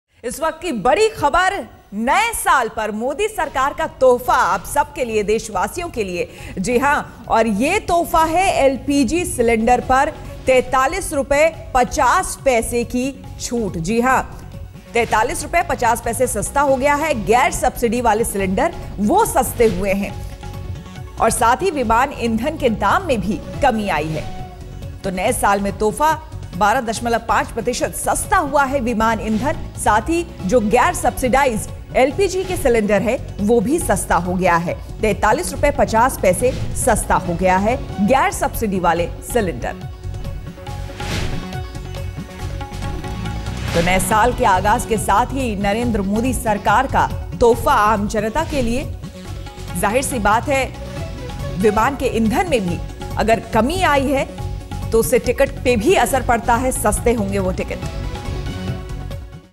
LPG cylinders cheaper Rs.43.5, jet fuel by 12.5%
The new year begins with good news for many as non subsidised LPG cylinders are to be cheaper by Rs.43.5. Jet fuel is also set to be made cheaper by 12.5%.